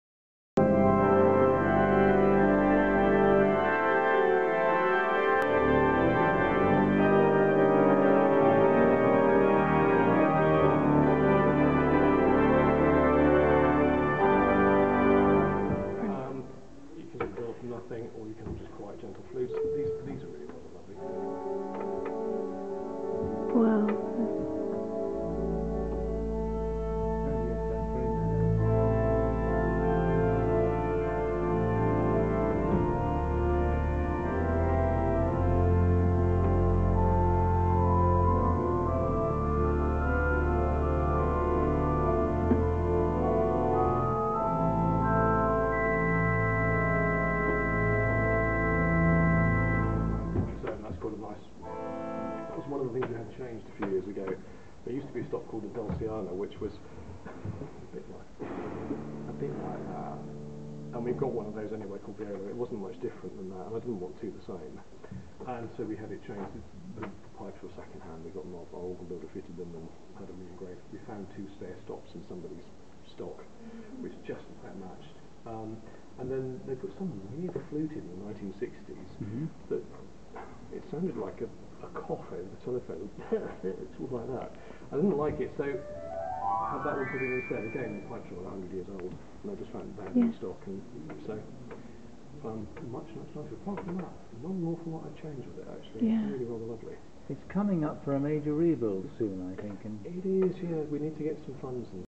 Organ Wimborne Minster, Dorset, UK
Sean Tucker, resident organist at Wimborne Minster, Dorset, demonstrates the organ that David Whiting's great-grandfather, Edmund Barnes, probably learnt to play on while a chorister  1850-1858.  From 1869 to 1882 he was organist at St. Clement Danes in the Strand, London.  (Bombed in 1941, now rebuilt it is the central church of the RAF where Lord Dowding’s statue stands.)